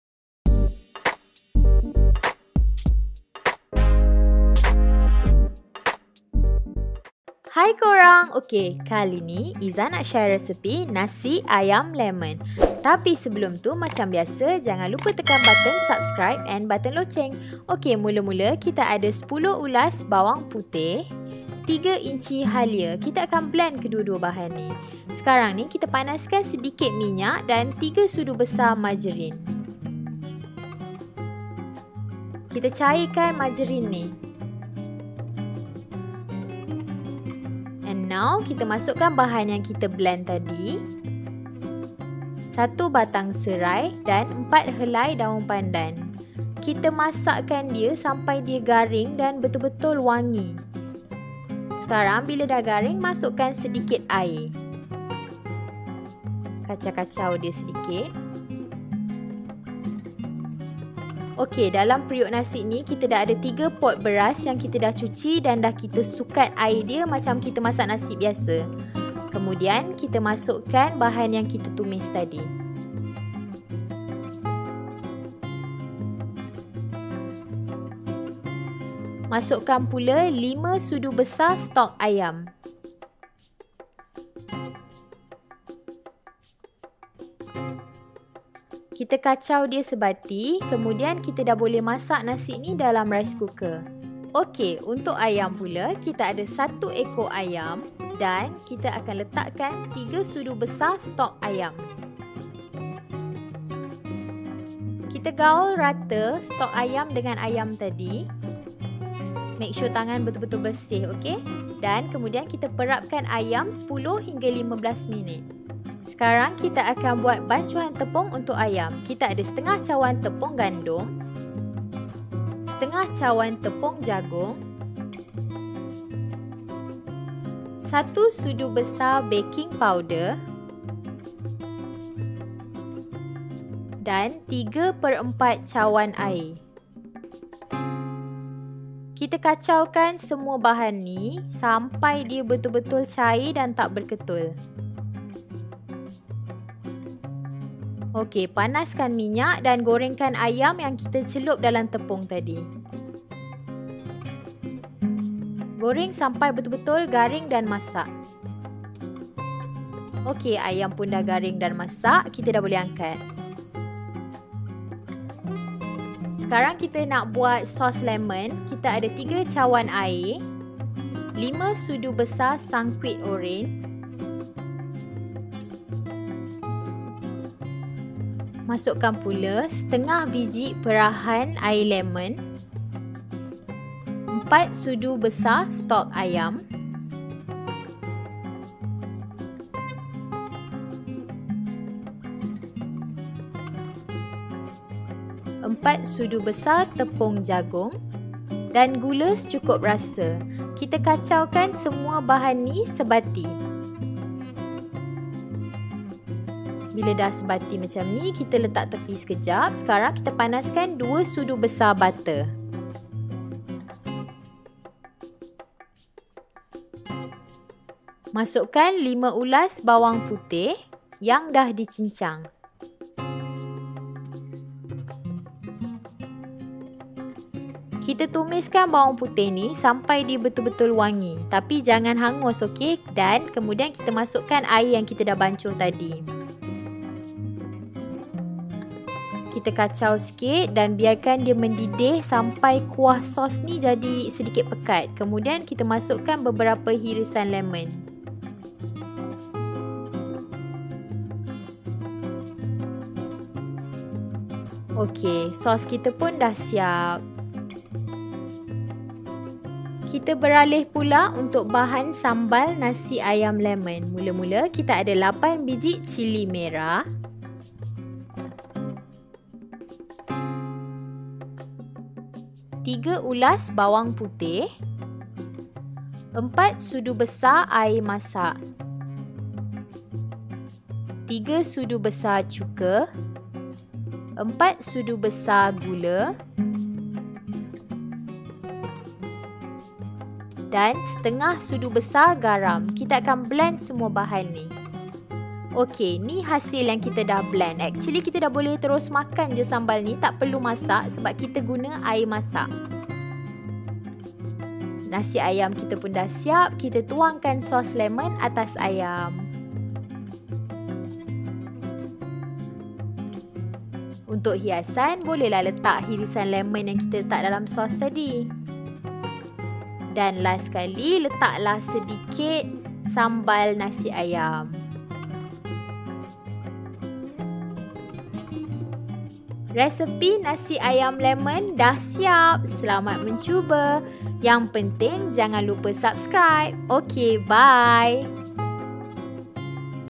NASI AYAM LEMON ALA PAKLI KOPITIAM
Hai korang! Jangan lupa cuba resipi Nasi Ayam Lemon ni ok!..

Untuk resipi n cara2 memasak boleh tengok kat video.